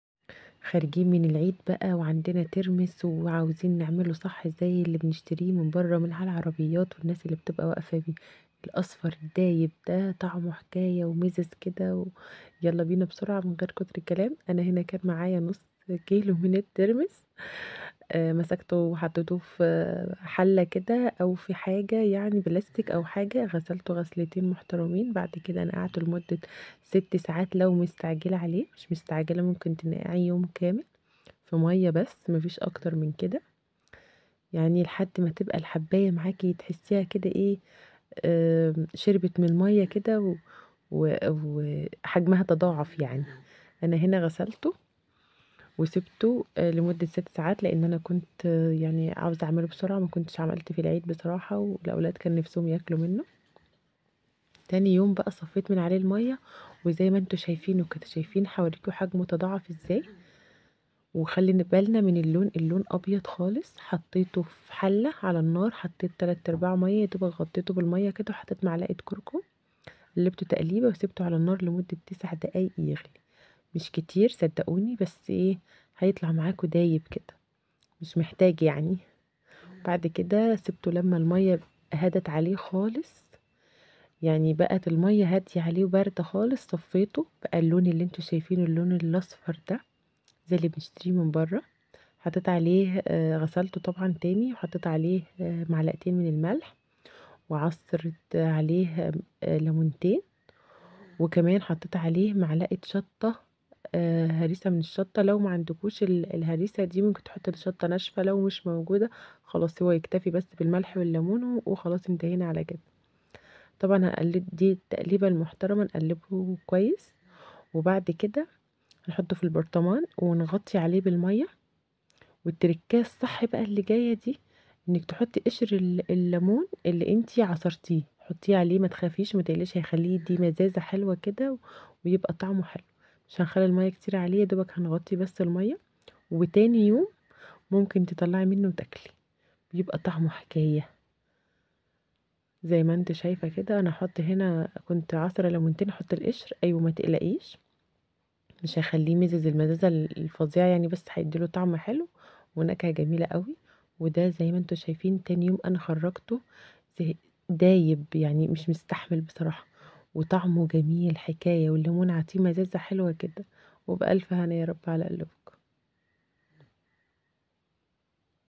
الترمس المشطشط بكل تكاته وحركاته ولونه الاصفر من حلوته هيتاكل ف ثواني
ترمس
كوركم
ماء
ملح
شطه
هيكون عندنا احلي ترمس
الطريقه نقع الترمس لمده 12 ساعه و لو مستعجله عليه يبقى ست ساعات نصفي  الماء من عليه ونبتدي نحط ميه جديده وعلى النار ثلاثه ارباع الحله ميه تحط فيها الترمس ومعلقه كوركم يغلي غلوتين تلاته نصفي ماء العلي ونضيف معلقتين ملح ومعلقه من معجون الشطه وعصره لمونتين ونضعه في برطمان محكم الغلق ونأمل منه تاني يوم